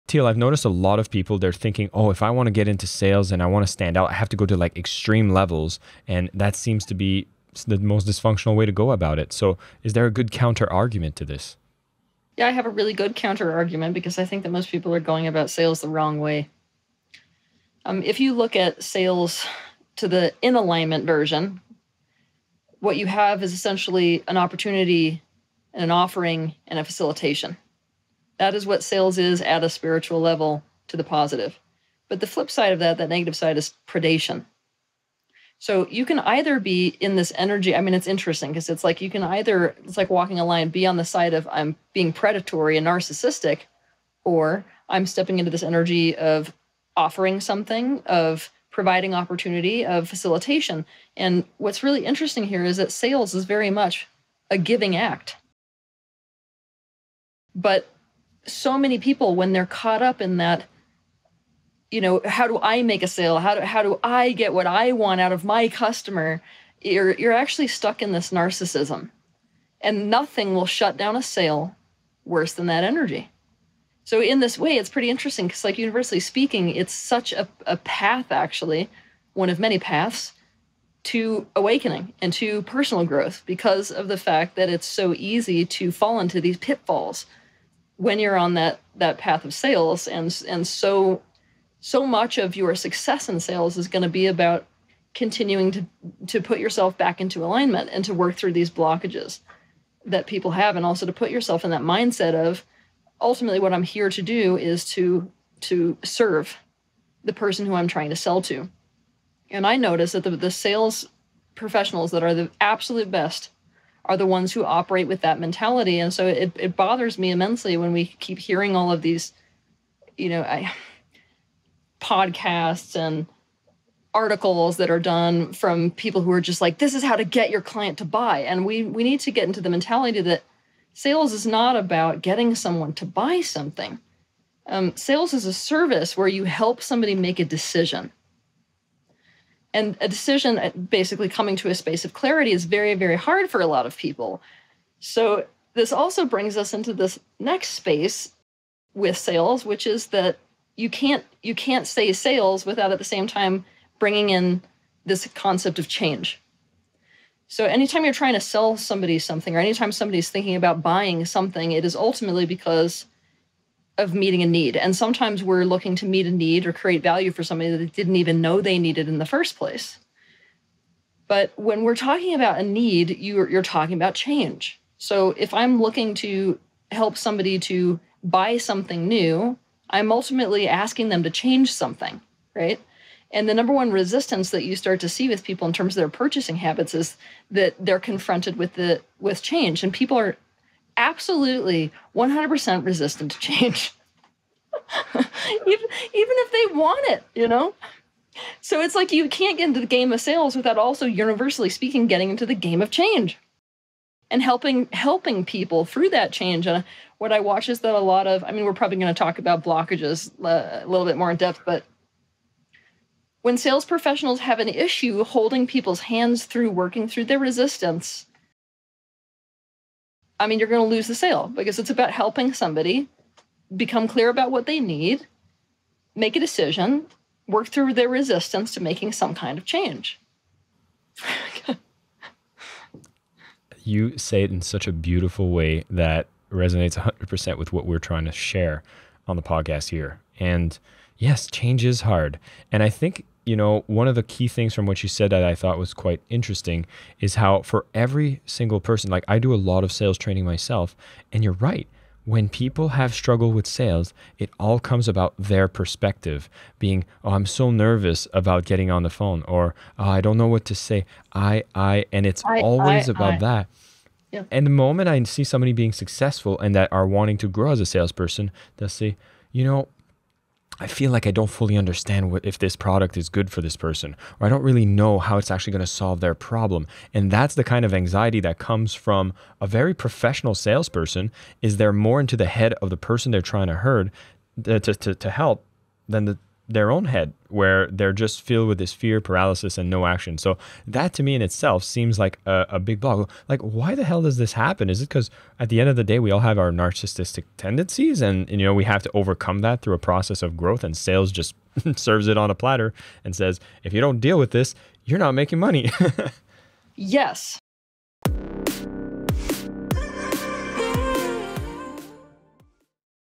The MOST Important Sales Tactic | Teal Swan
❤️ Free Mini Course - How To Identify, Attract & Close Ideal Clients
In this clip, Teal Swan shares the most important sales tactic to help you get more sales for your business. Teal, shares why sales is very much a giving act and why so many people get it the wrong way.

🎙️Guest’s Bio
Teal Swan is an American spiritual influencer and author of internationally published books and the “Ask Teal” YouTube series. She’s also the founder of the Headway Foundation, a nonprofit company that enables ideas, goals, and ventures that aim toward positive world change.

Teal travels the world teaching about the truth of the universe, helping people find the truth of themselves and transforming their emotional, mental, physical, and spiritual pain. Using strategic search engine optimization, she has built a strong following on most social media platforms.

❤️  Popular Selling With Love Episodes

❤️ New to Selling with Love Podcast?
Download the first chapter of the Selling With Love book FOR FREE